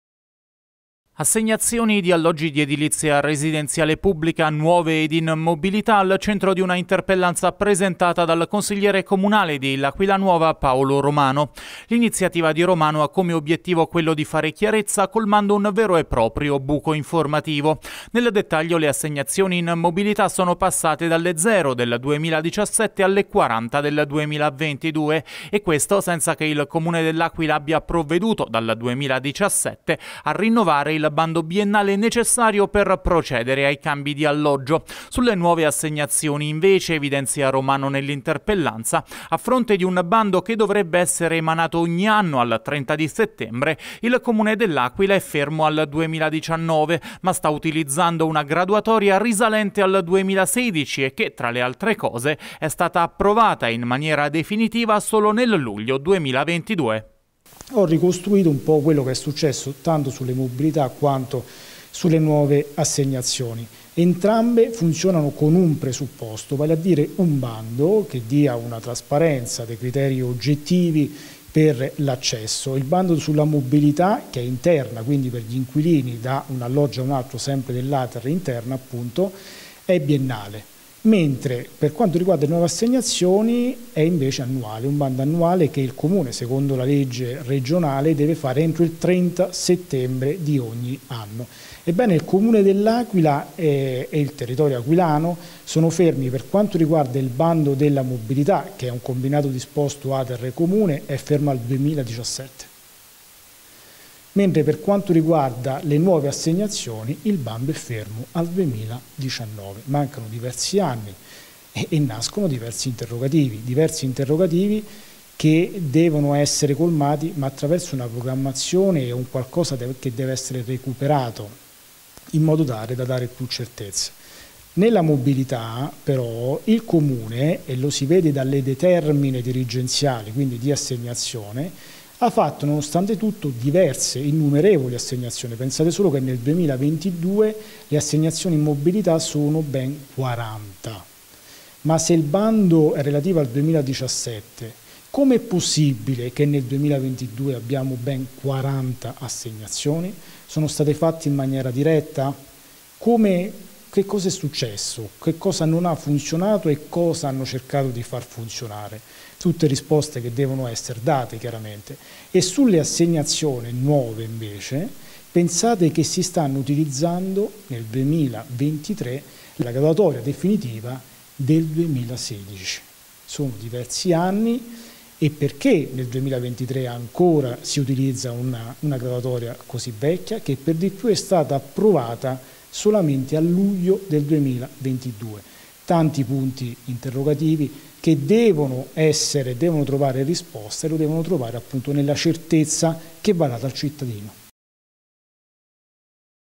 ERP ROMANO LAQUILA NUOVA CHIAREZZA SULLE ASSEGNAZIONI NUOVE ED IN MOBILITA
INTERPELLANZA DEL CONSIGLIERE COMUNALE DI OPPOSIZIONE PAOLO ROMANO.

L'ESPONENTE DI L'AQUILA NUOVA CHIEDE CHIAREZZA SULLA GESTIONE DEI BANDI PER LE ASSEGNAZIONI DEGLI ALLOGGI DI EDILIZIA RESIDENZIALE PUBBLICA